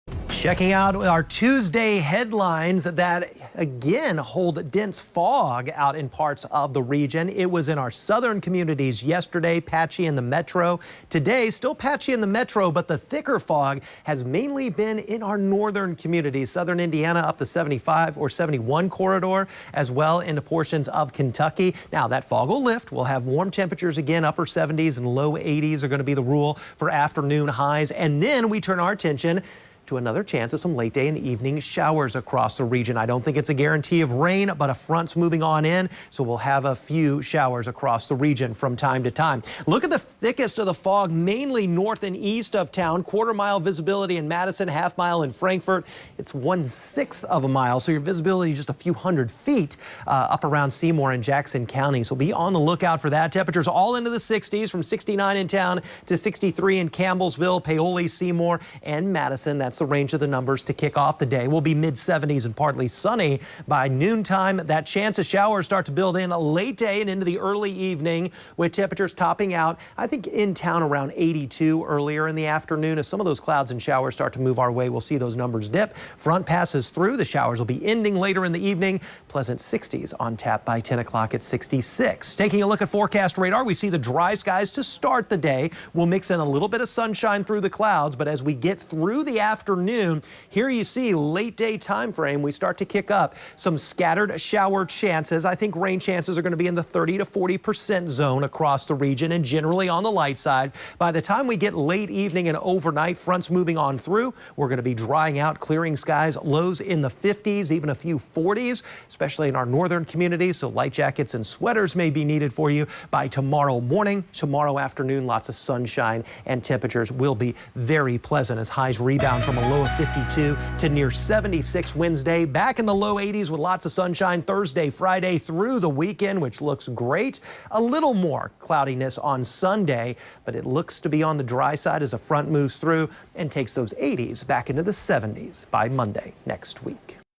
Warm, with late day shower chances Tuesday
WLKY Meteorologist Matt Milosevich says, we have one more chance of showers, before drier skies move in to the 7-day forecast.